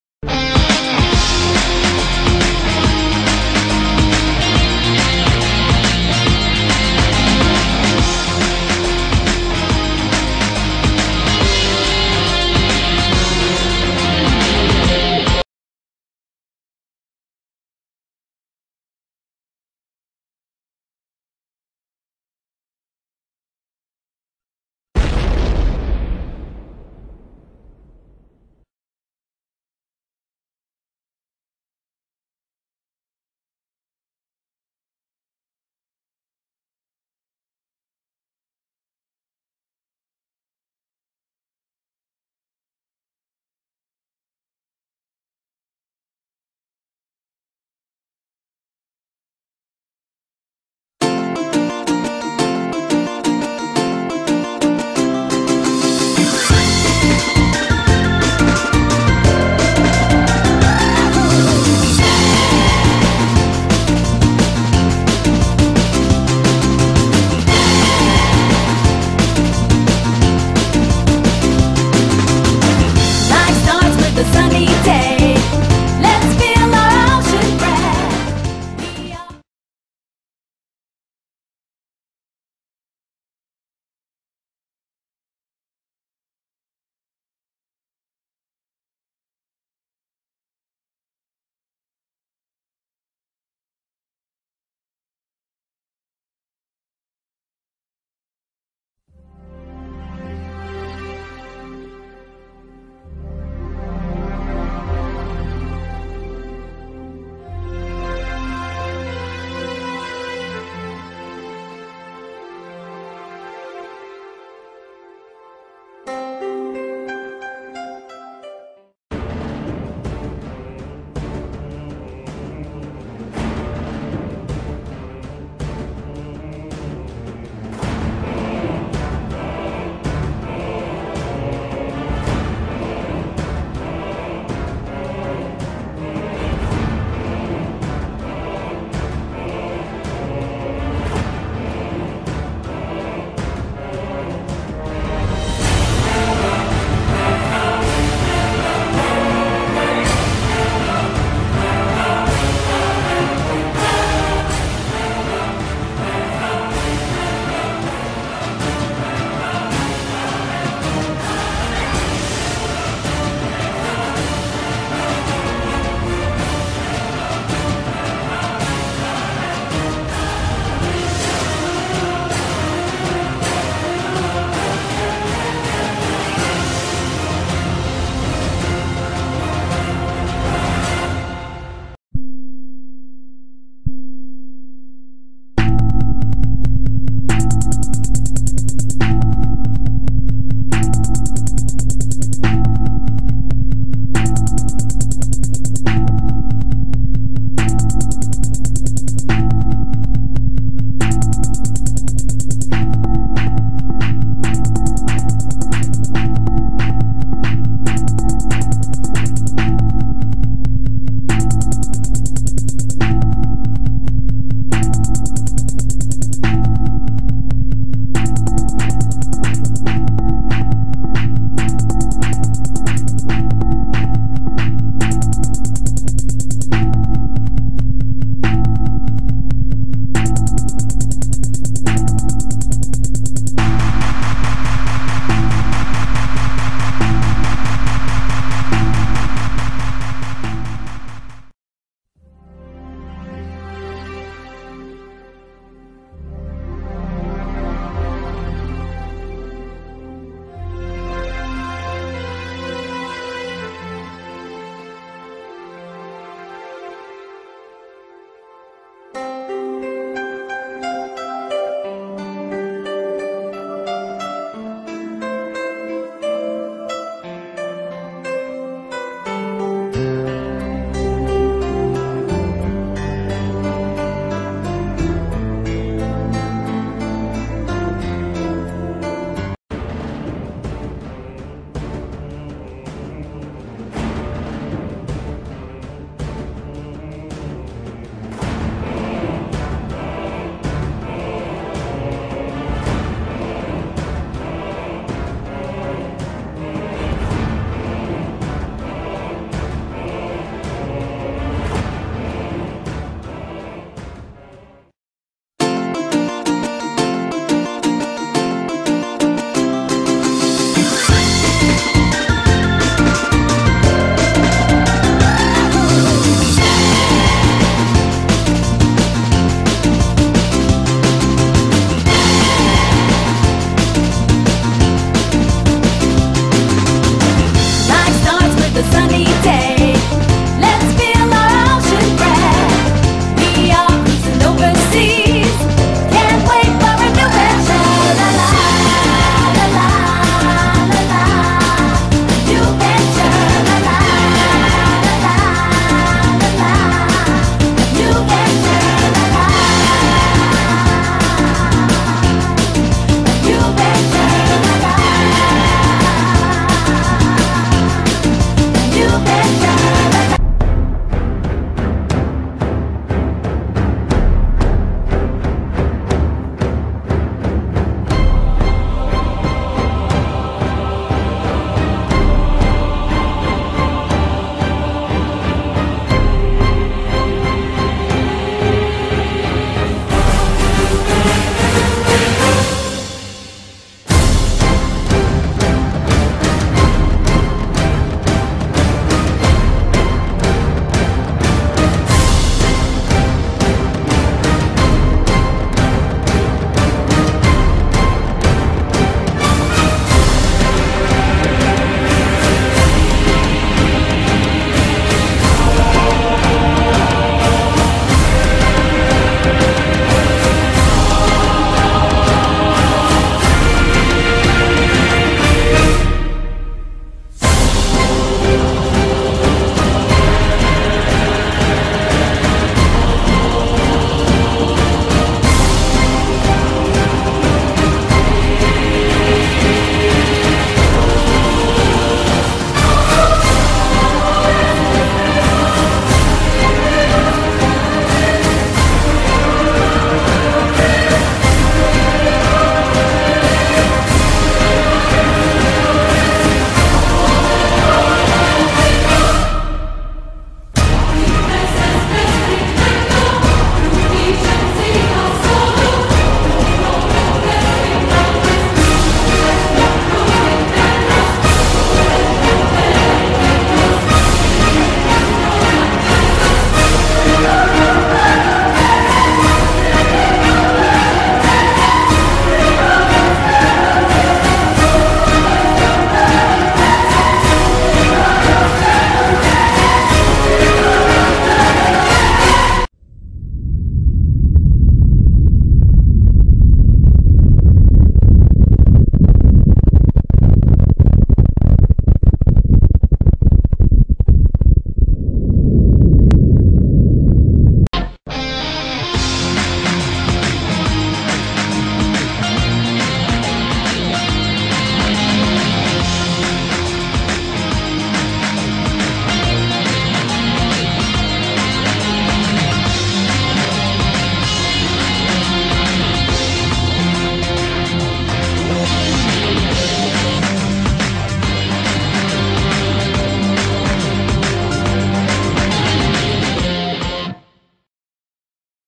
Sonic Sprites S2EP7 Betrayal Part 1/2
After many months away from Mobotropolis, King Acorn, Mina, Ash, NICOLE and Rotor return! But what does the King have in store for Sally?